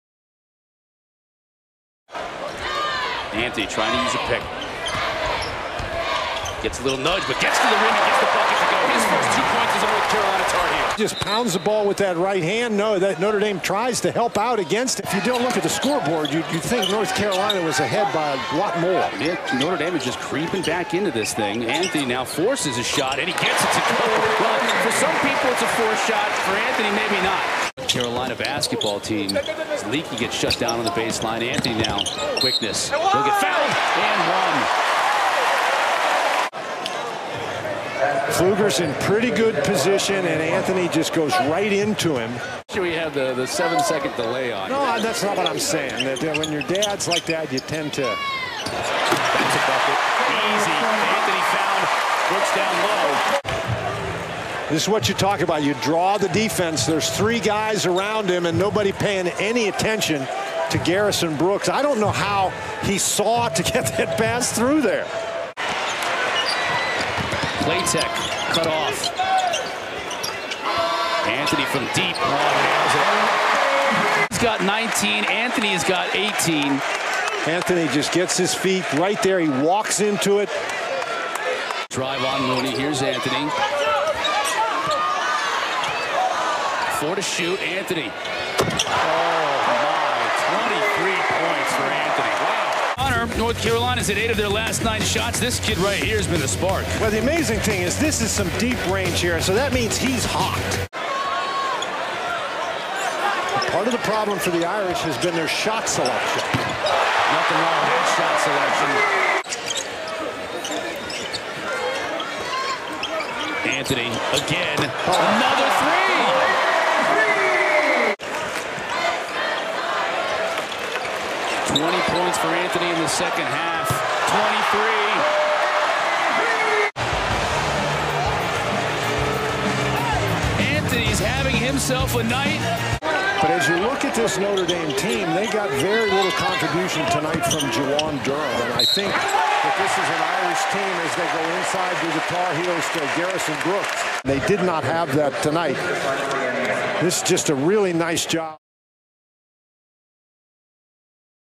UNC Freshman Cole Anthony Sets Record in Debut
UNC star freshman Cole Anthony lived up to his billing in his debut with the Tar Heels.  Anthony set a new school record for freshman scoring in their first game with 34 points on 12-of-24 shooting, had a game-high 11 rebounds and tied for the team lead with 5 assists.  Anthony wowed the home crowd as he routinely hit big shots as the Tar Heels rallied from a second half deficit to win the game, 76-65.